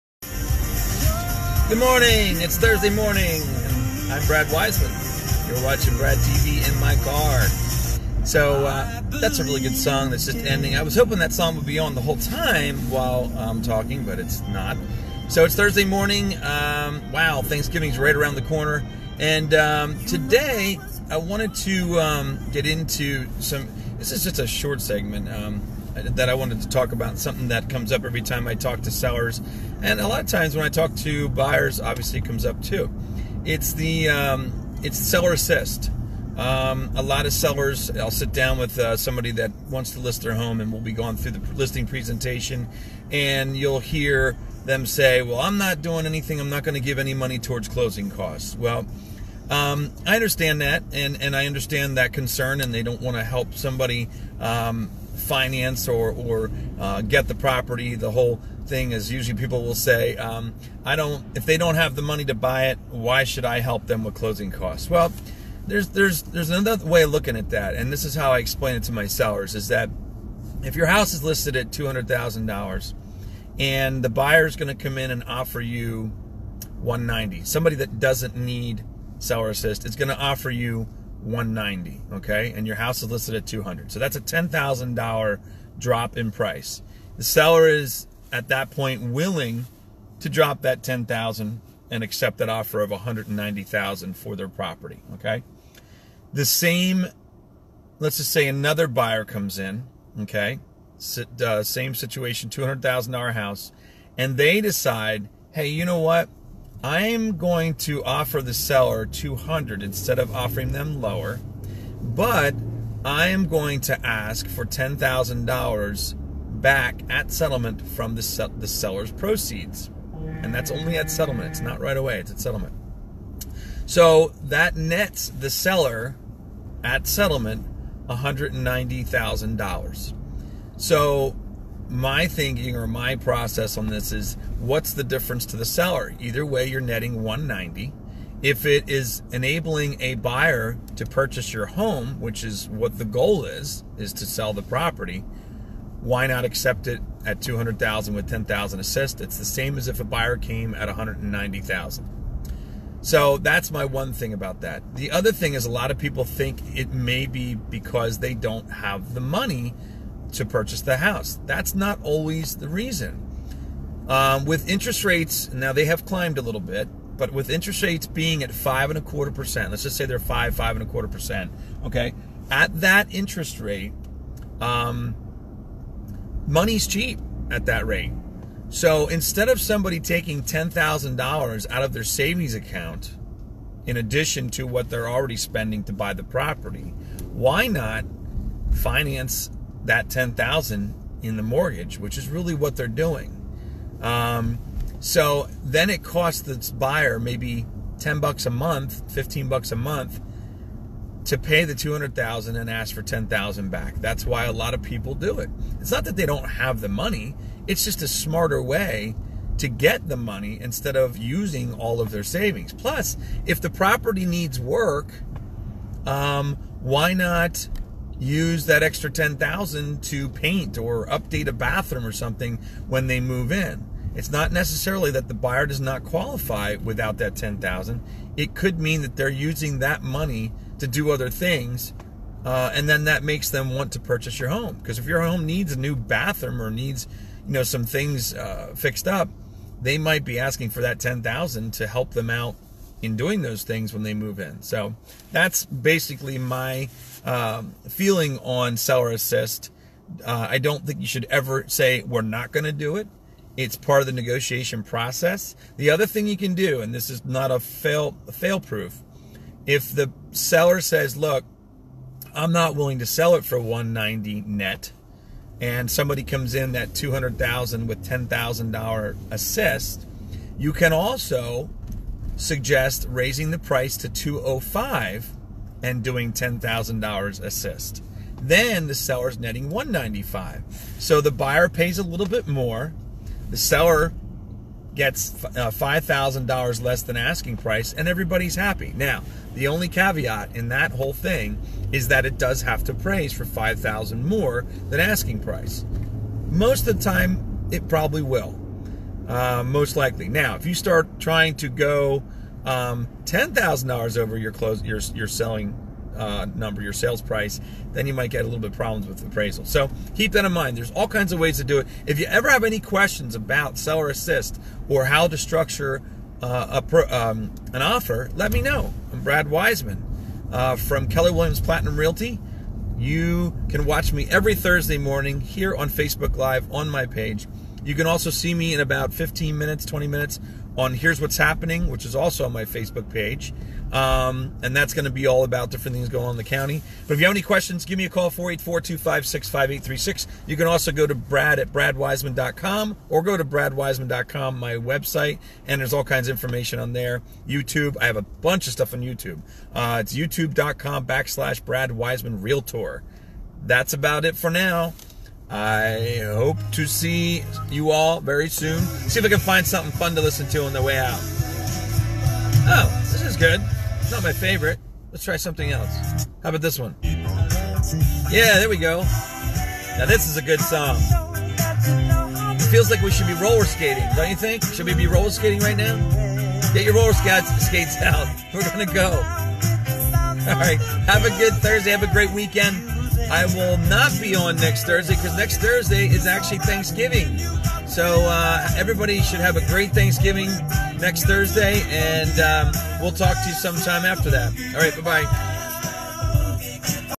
Brad TV - Seller Assist YES or NO
Sellers tend to not like hearing about Seller Assist... but really it all cones down to the NET offer. Listen to my explanation on why Seller Assist is nothing to worry about.. it's all part of negotiating the price. It all comes down to what you walk away with at settlement.